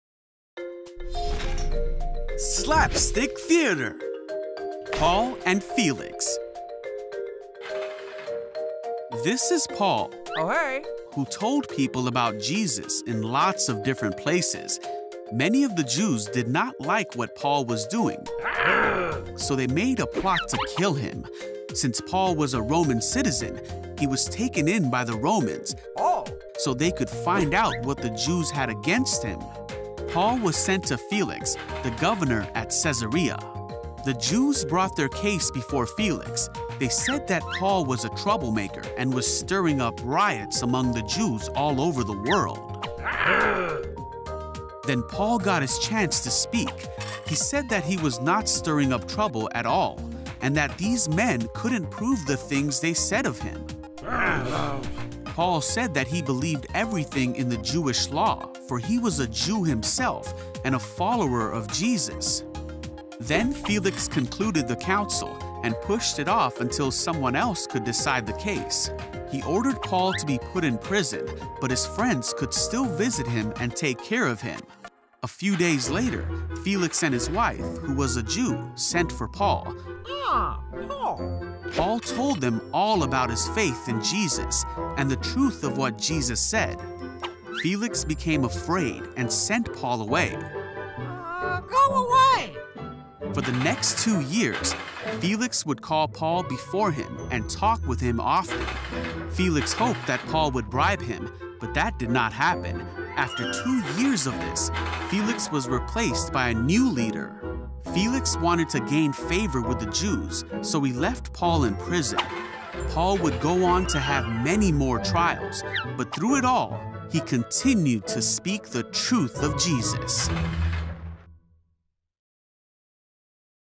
Slapstick Theater (Paul and Felix)
This animated Bible story is based on Acts 24.

We tell the story through Slapstick Theater - a popsicle stick theater.